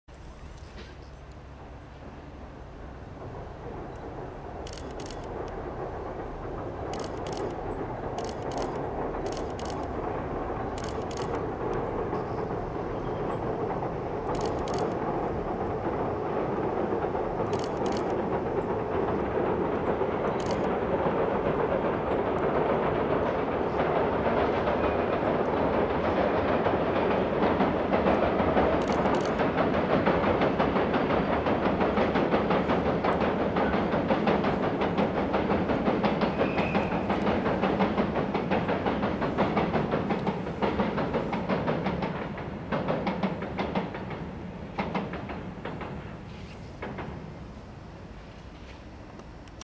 331 新宮時代の紀勢貨物凸 4
荷を積み、熊野川を渡って新宮に戻ります。
2008年2月